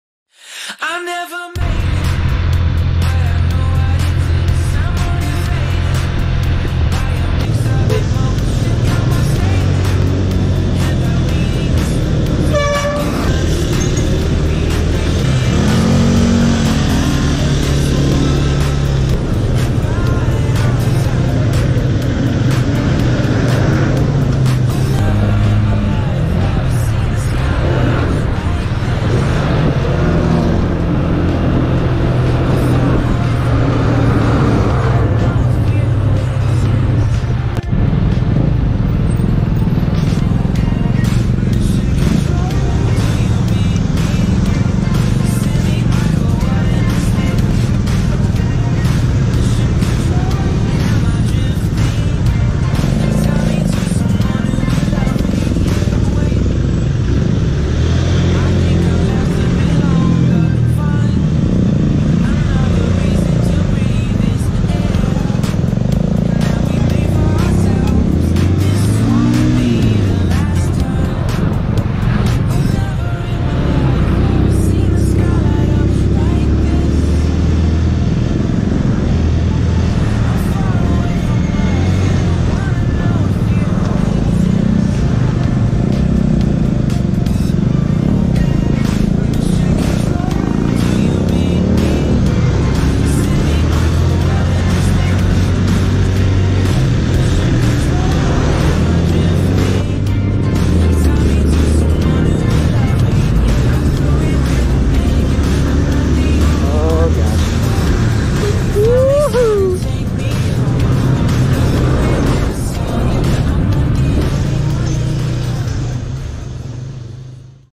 Traffic Infractions
Would you ride on the back? Traffic can be crazy in Antananarivo, but a motorcycle can make the commute a fraction of the time.